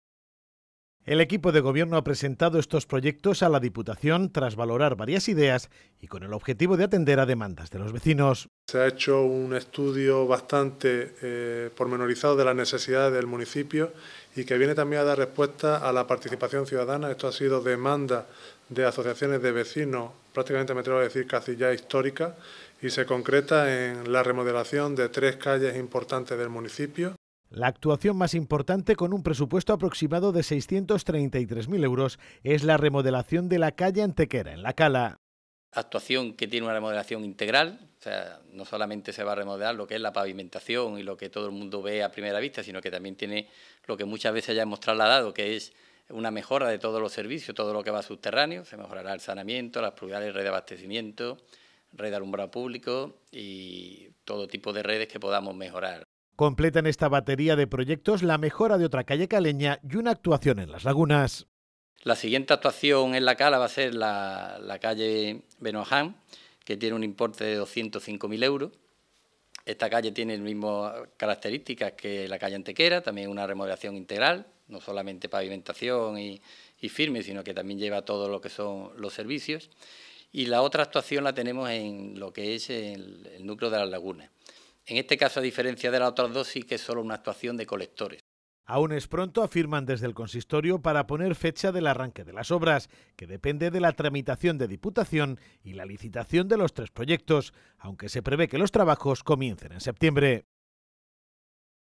Arreglo de calles
La Diputación sufraga la remodelación de las calles Antequera y Benaoján, en La Cala y la calle Virgen de la Paz, en Las Lagunas. Las tres actuaciones. El consistorio ha propuesto al ente supramuncipal estos tres proyectos, que tienen un presupuesto total de más de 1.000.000 euros, para que sean incluidos en el plan de inversiones financieramente sostenibles de la entidad